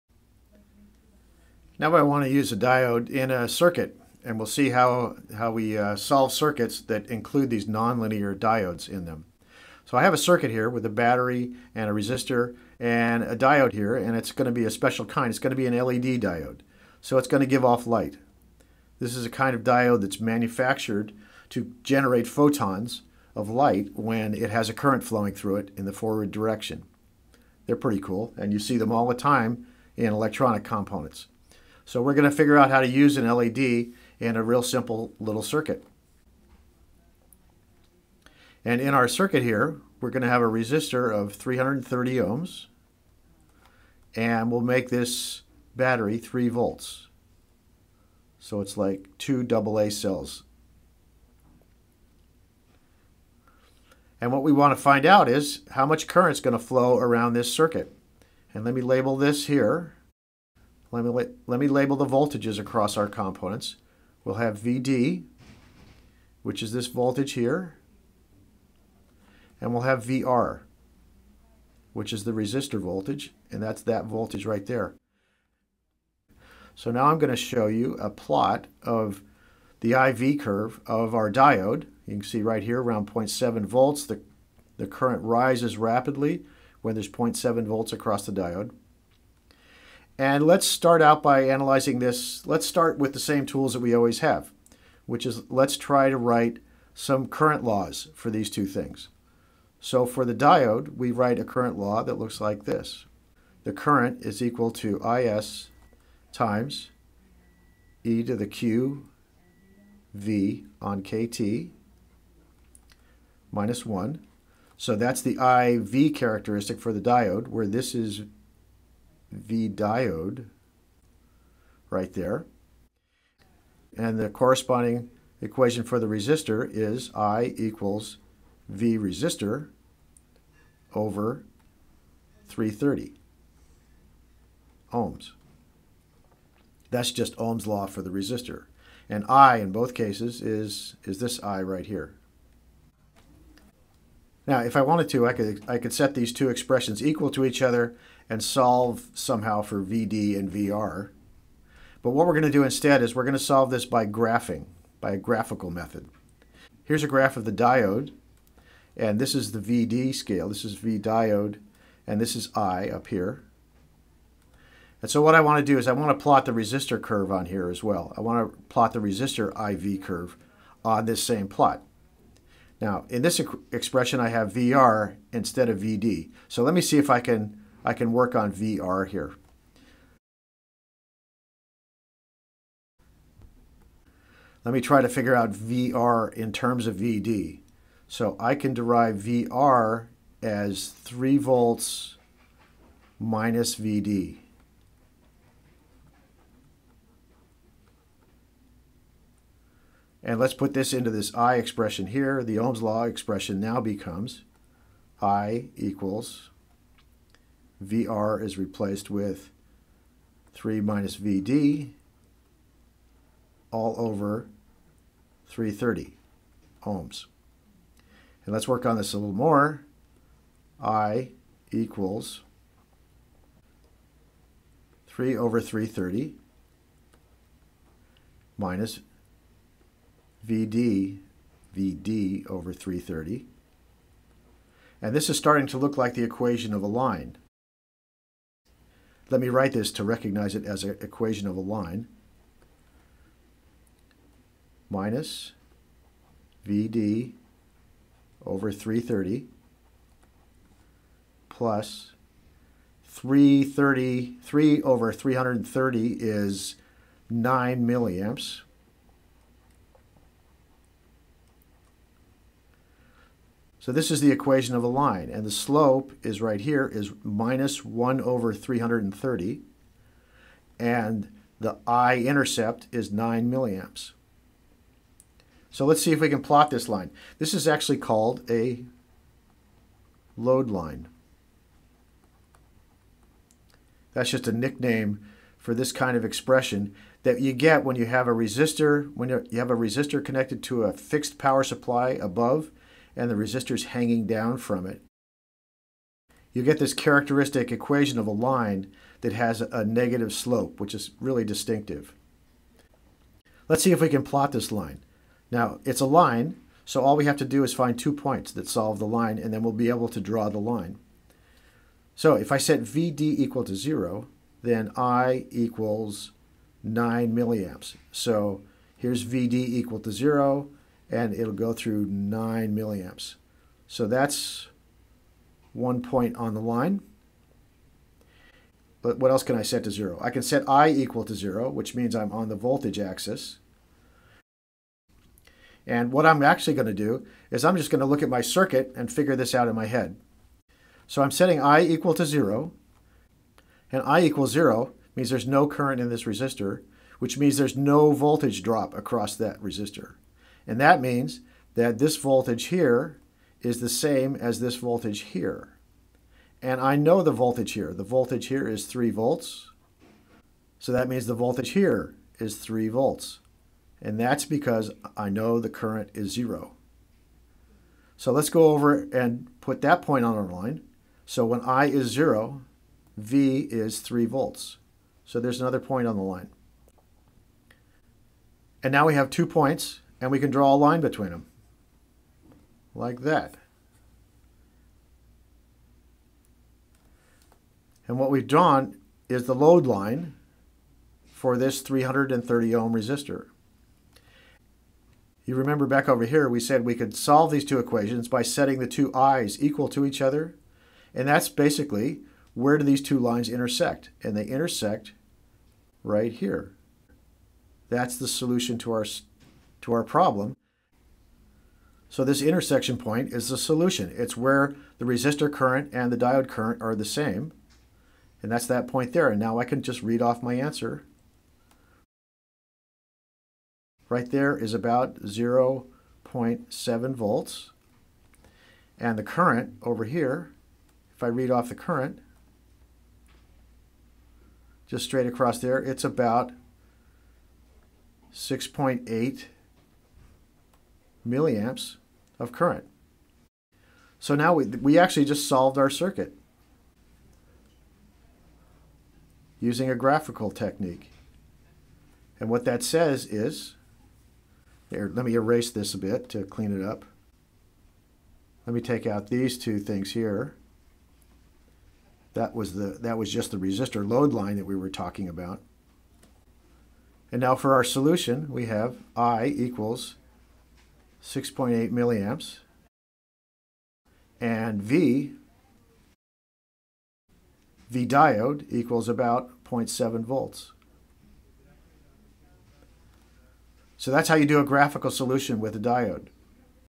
Diode graphical solution
We solve a diode circuit graphically by plotting a diode i-v curve and resistor to find the intersection. Graphical solution methods are a common way to work with non-linear components like diodes.